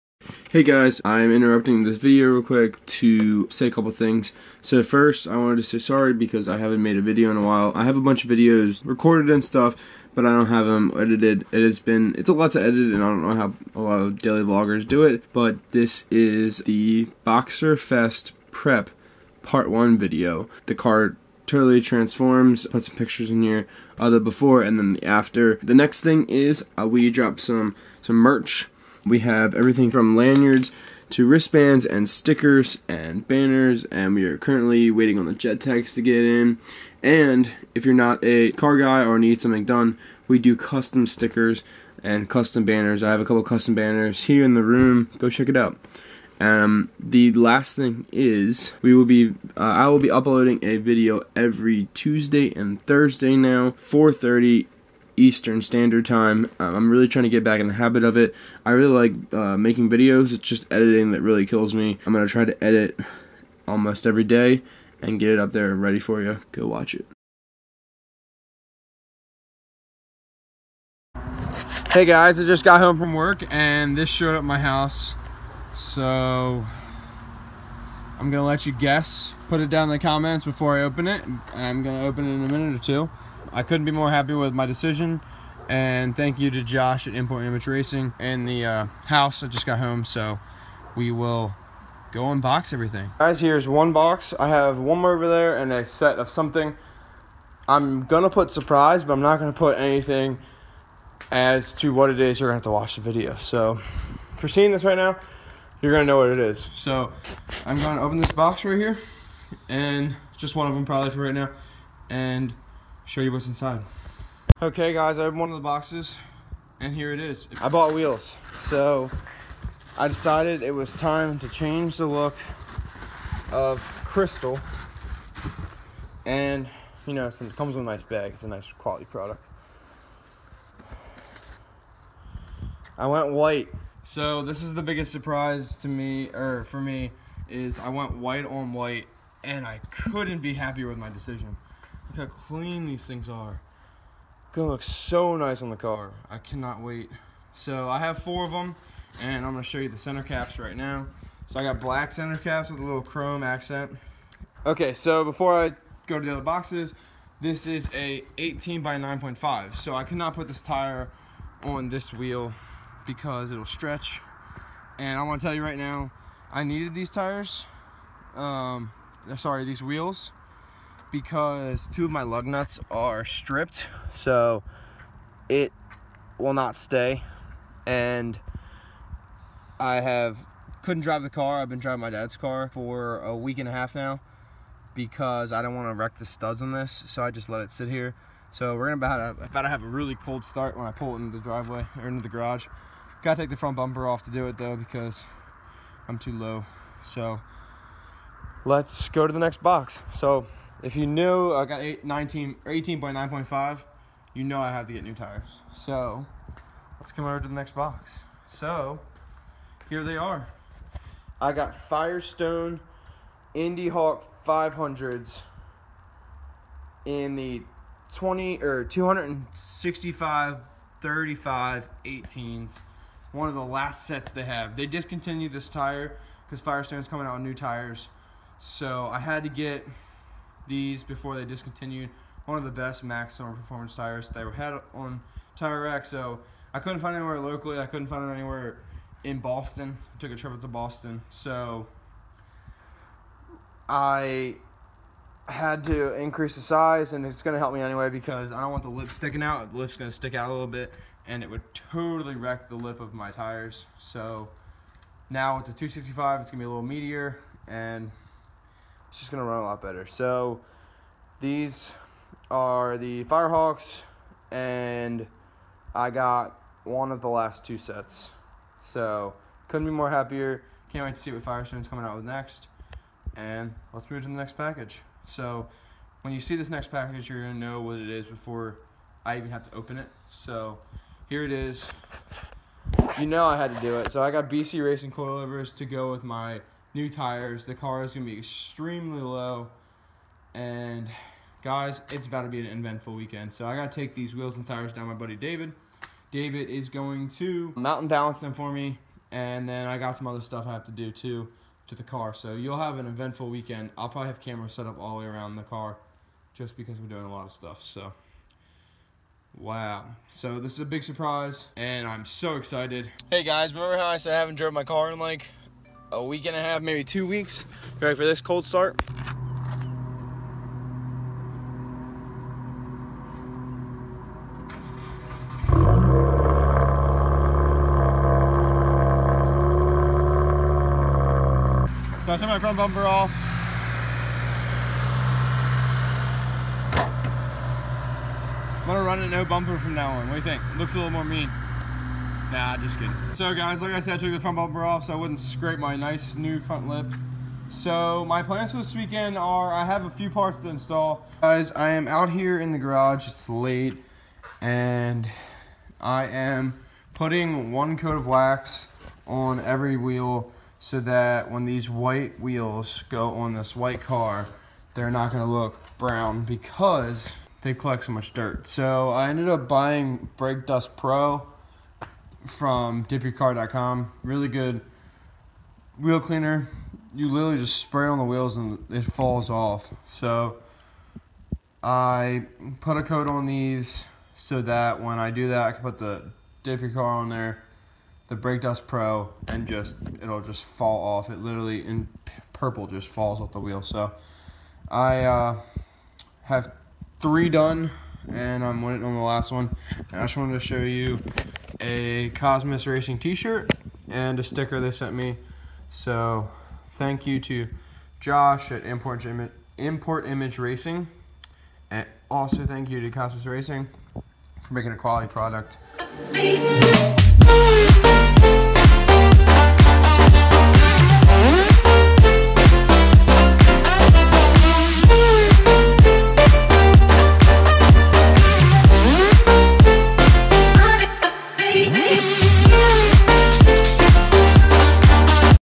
BOXERFEST PREP - UNBOXING!! (Part 1)
Boxerfest is a huge event and I wanted to totally transform it! This is the just the unboxing video and there are 2 more to go!

SEND ME STUFF:
Fallston, MD 21047

Parts on the Car:
Light Bar: